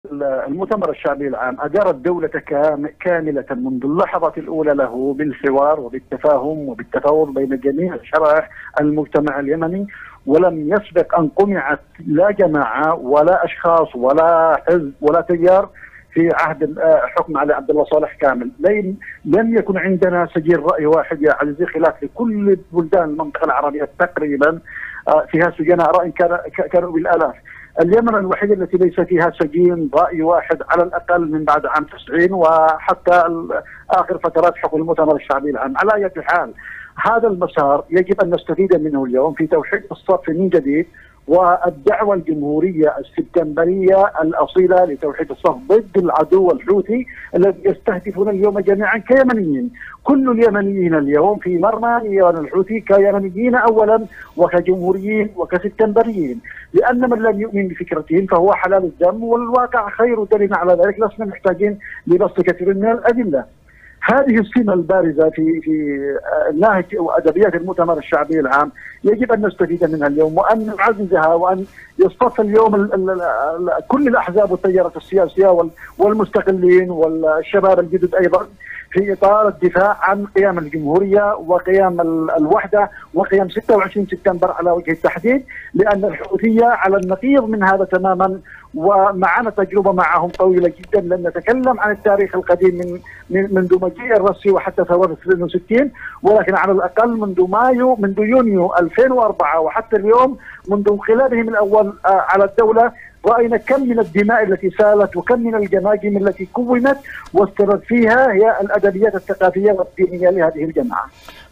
د/ ثابت الأحمدي: المؤتمر أدار الدولة بالحوار والشراكة وتقتضي الضرورة الوطنية الاستفادة من تجربته
المحلل السياسي د/ ثابت الأحمدي: المؤتمر أدار الدولة بالحوار والشراكة وتقتضي الضرورة الوطنية الاستفادة من تجربة المؤتمر لاستعادة مؤسسات الدولة